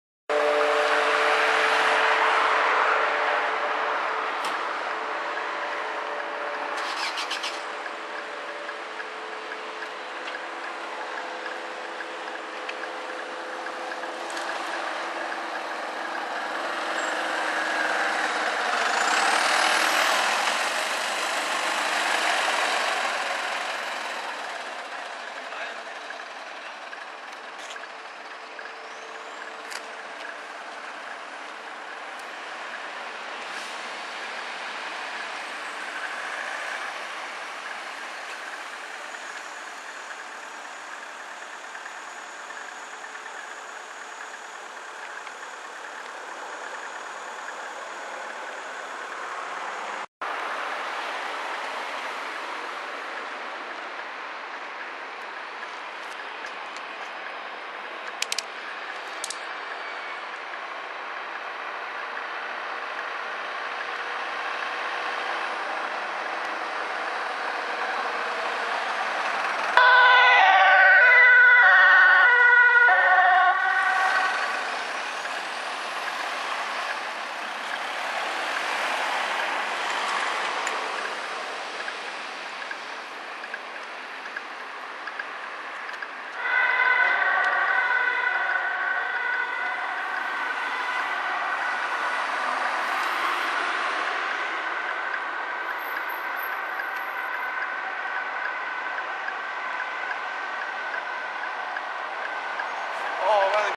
13-04-12 20:54 A1 17-302 micu naar 2 steden zkh tilburg aankomst + vertrek naar andere zkh
ze werden eerst met A2 opgeroepen maar kwamen met spoed aan